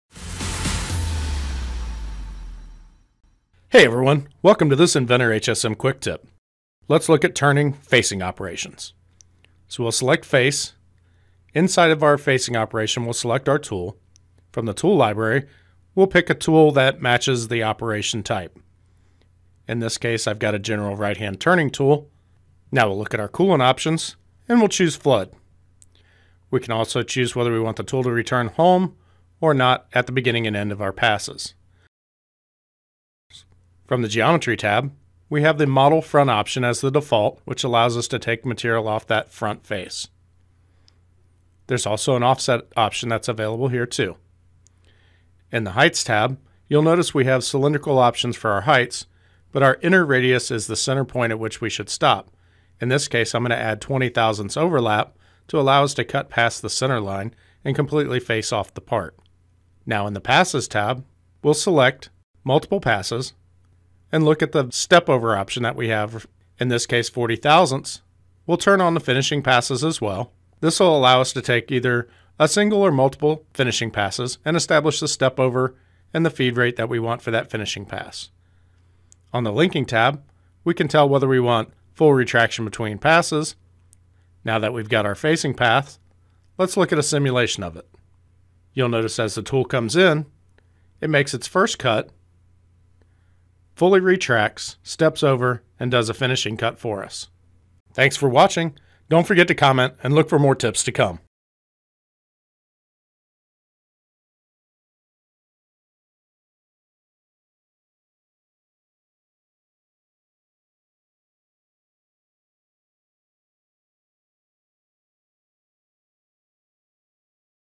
Inventor HSM QUICK TIP: Turning Face Operation
This Quick Tip takes you through each parameter of the Turning Face Operation, so you can better control each section of your toolpaths.